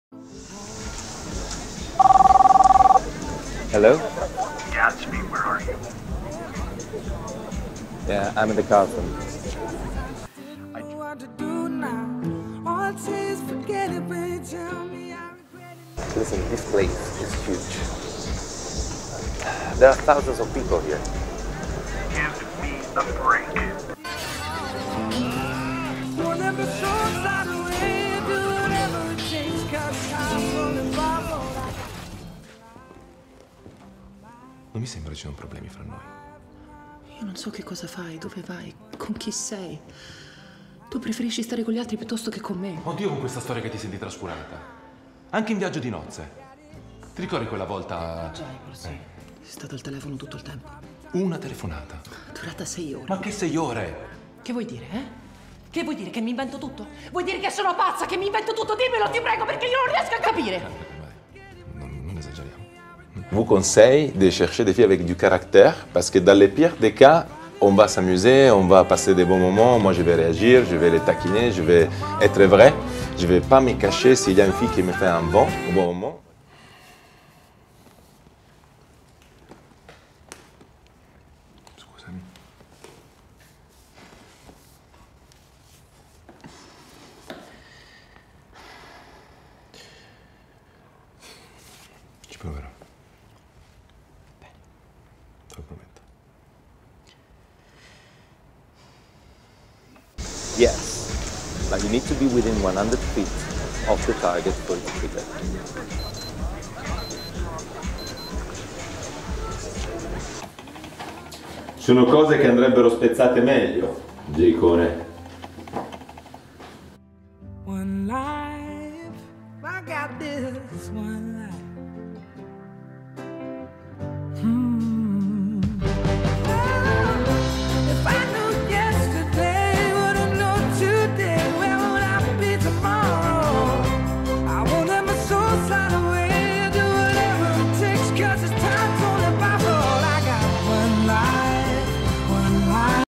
2'Reel Gian Marco Tavani
Last demo video in English, French and Italian from the italian actor Gian Marco Tavani. Find more in